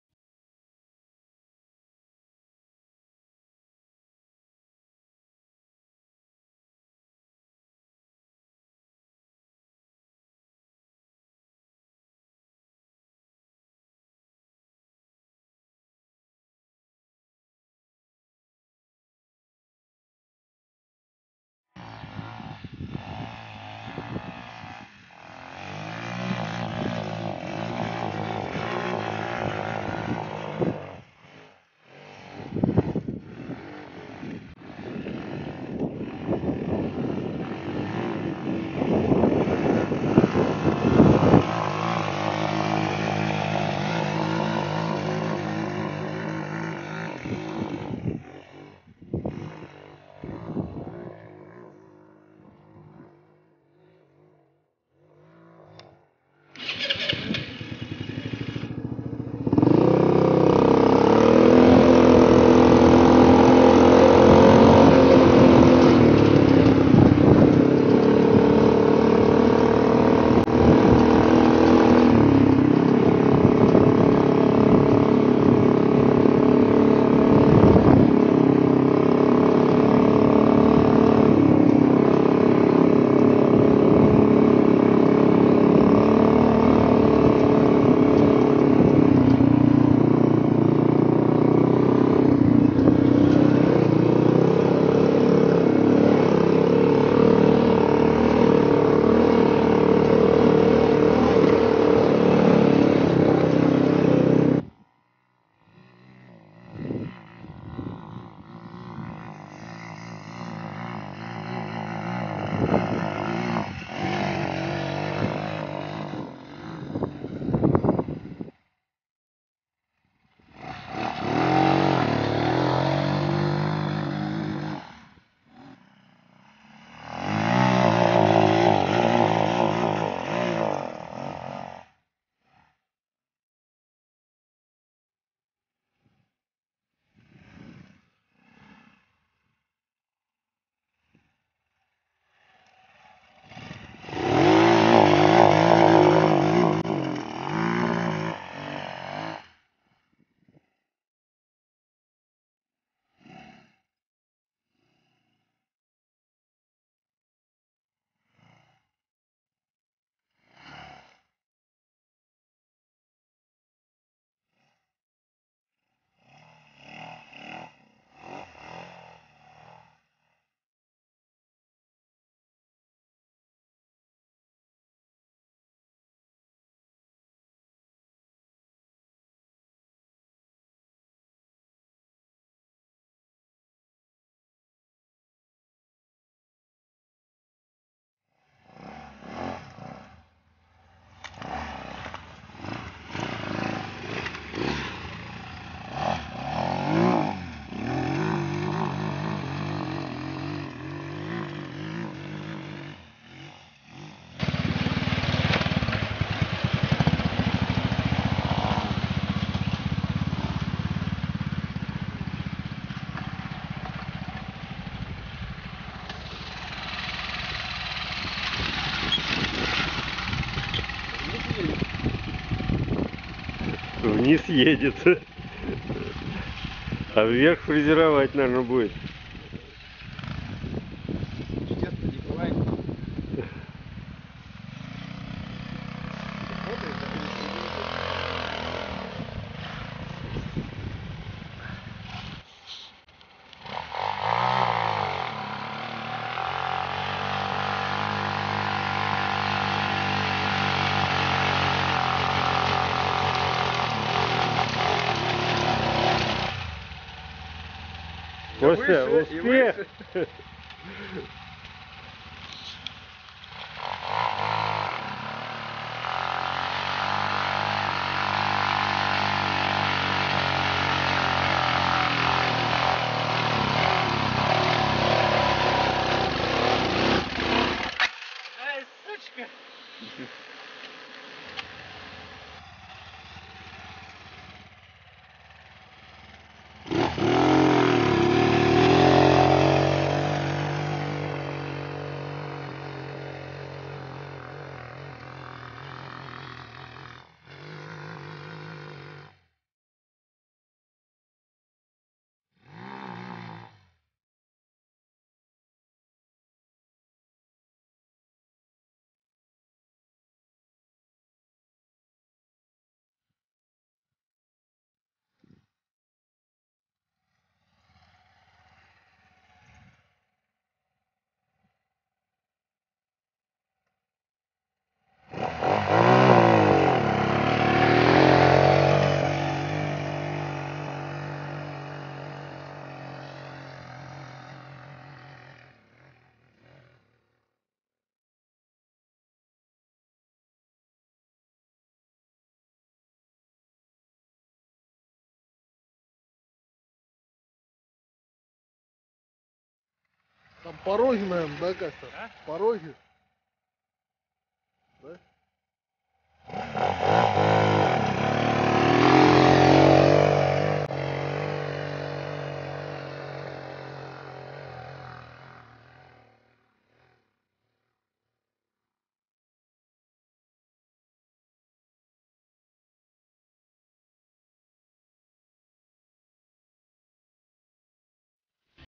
Самодельны квадроцикл 4х4 часть3
Двигатель 250сс 4+1
Блокировки в мостах
Отключаемый передний мост